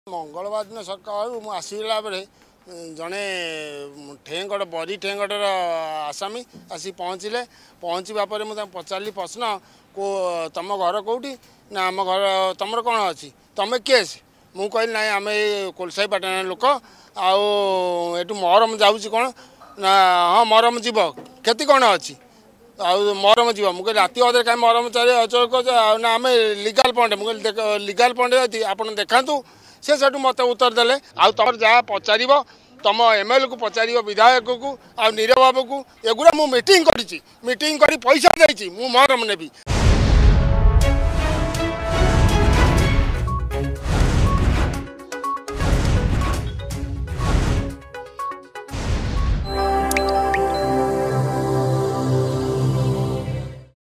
ବଡଚଣାରୁ ଲଘୁ ଖଣିଜ ସମ୍ପଦ ଲୁଟ୍
ବଡଚଣାରେ ଖଣିଜ ସମ୍ପଦ ଲୁଟ୍ ନେଇ ଶୁଣନ୍ତୁ କଣ କହୁଛନ୍ତି ସାଧାରଣ ଜନତା.
ଅଧିକ ଖବର ପାଇଁ ଡ଼ାଉନଲୋଡ କରନ୍ତୁ “Argus News” ଆପ୍